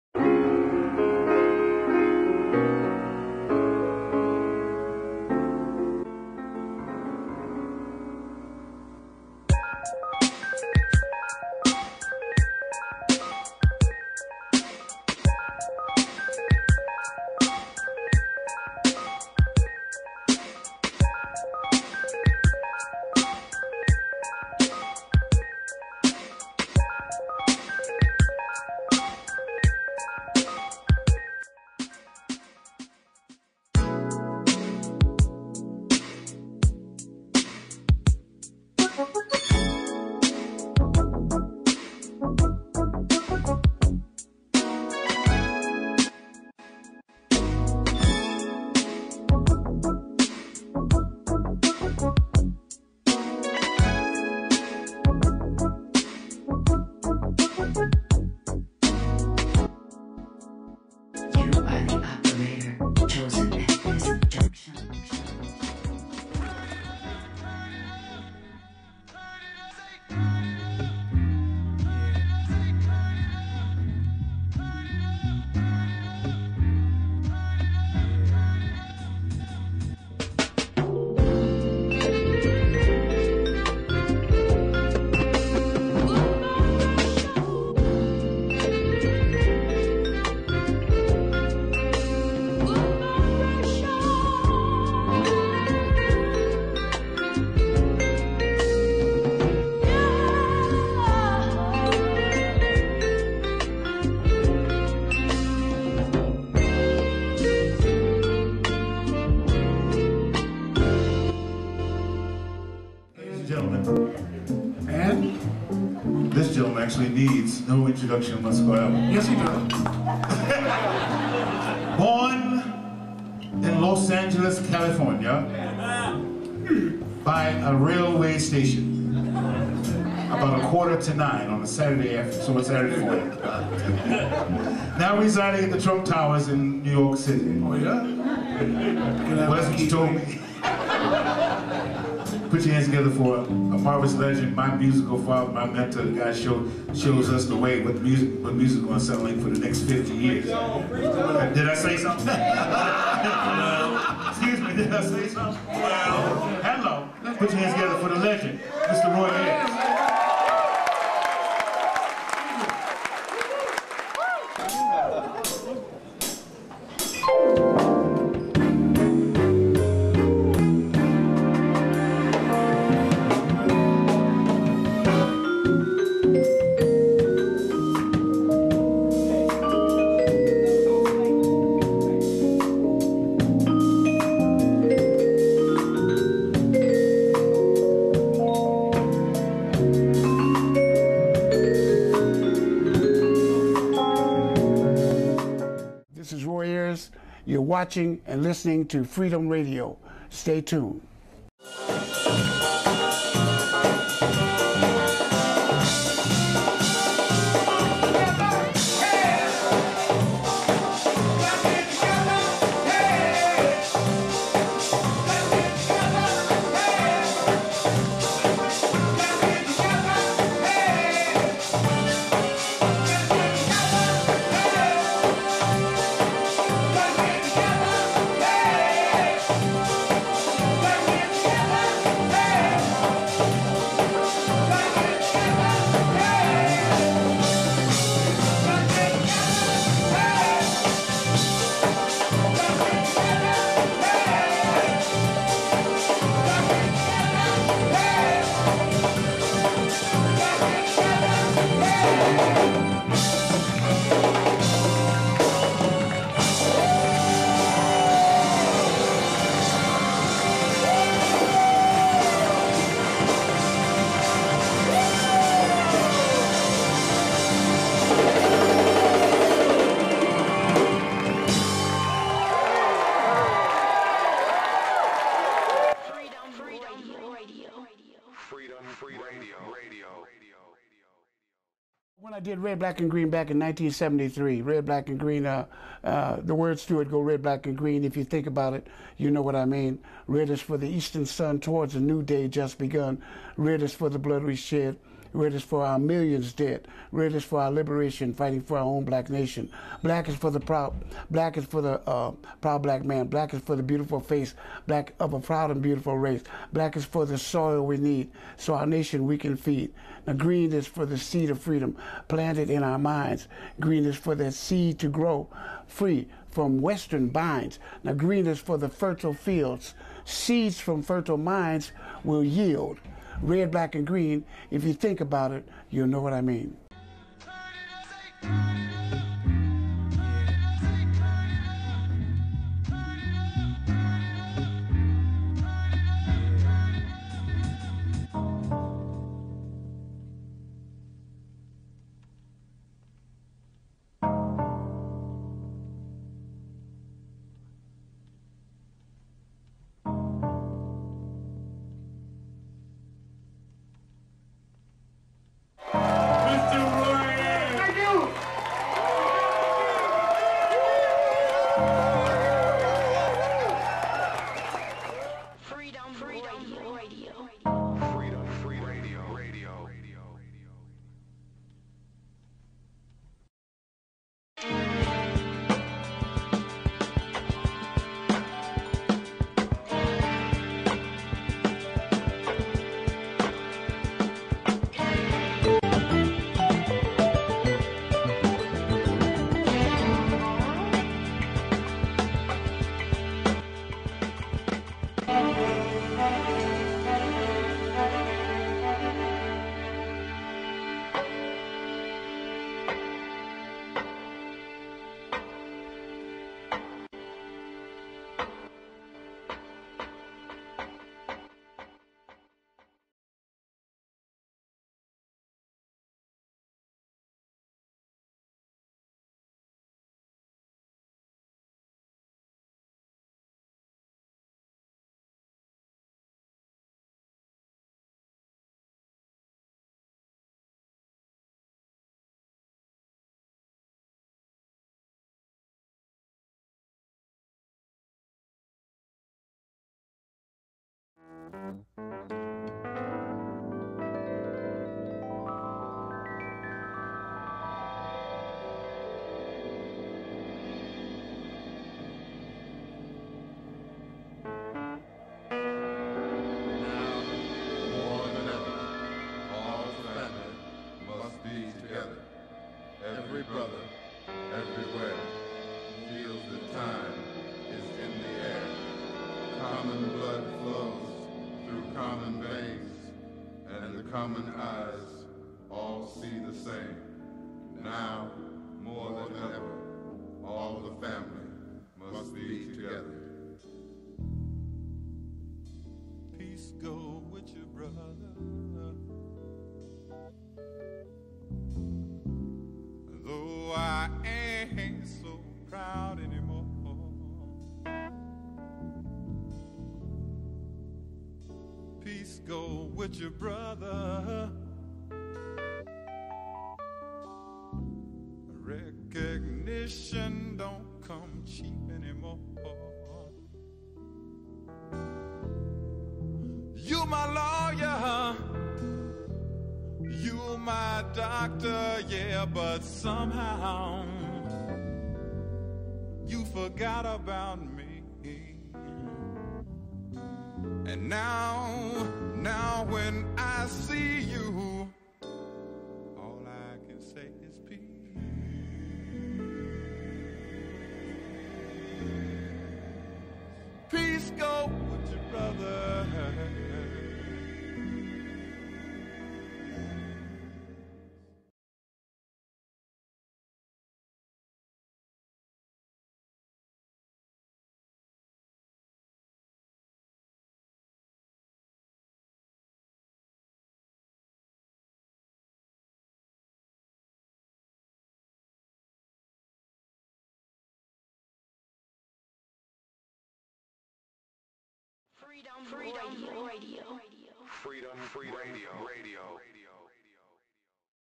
Freedom Radio Roy Ayers Jazz Concert Documentary... (Promo)
Exclusive Jazz Documentary Film on Roy Ayers and his impact on Black music and culture worldwide. Freedom Radio News & Culture Television Network invites you to join us at 5:30 p.m. Sunday, July 24th 2011 as we show this powerful music experience to our online audience. We will see you fresh in the place to celebrate this musical giant.